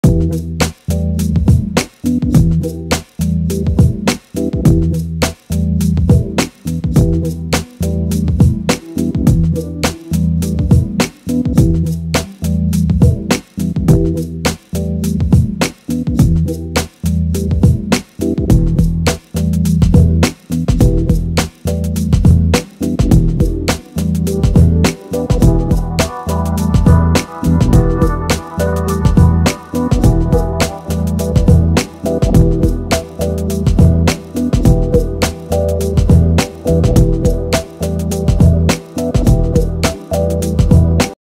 Ambassador hat - Russian dark brown beaver fur, black leather top
Introducing the Ambassador style hat in natural beaver fur. A beaver spends the majority of its time in river waters and its wool is most resistant to rain and snow. Even after a heavy snowfall, the fur retains its properties, you simply need to shake your beaver accessory a couple of times and it will be restored as in the picture. If the weather in your region is prone to heavy precipitation in autumn and winter, you should choose beaver fur accessories. The top of the hat is available in either black or dark brown leather (as shown in the video).